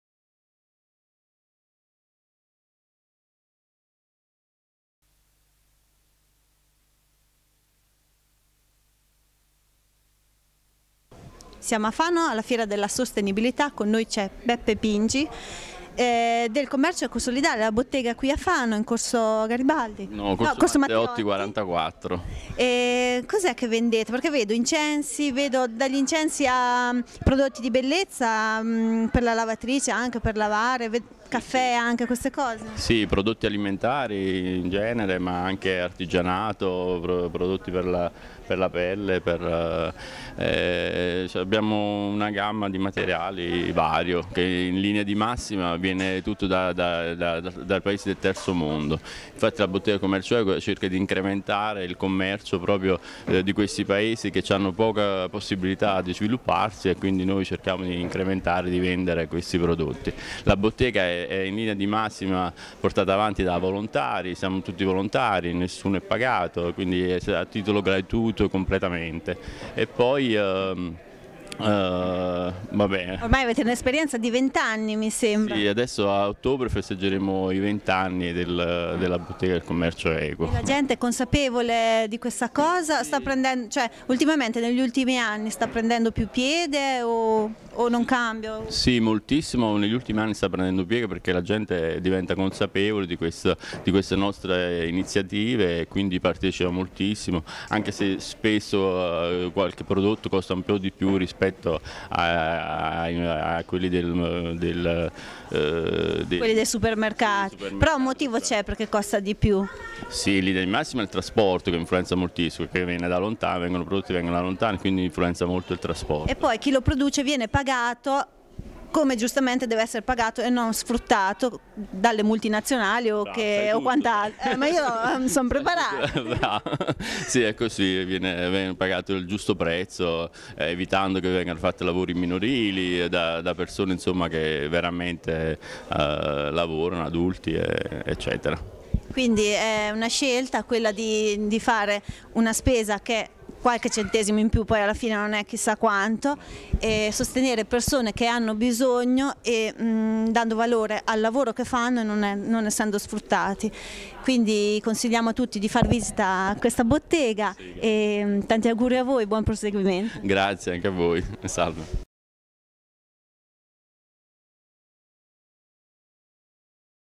commercio equo solidale
Alla fiera della sostenibilità abbiamo incontrato i volontari della Bottega del Mondo , per un commercio equo solidale,con sede a Fano.
Ci presentano un modo più responsabile nel fare acquisti.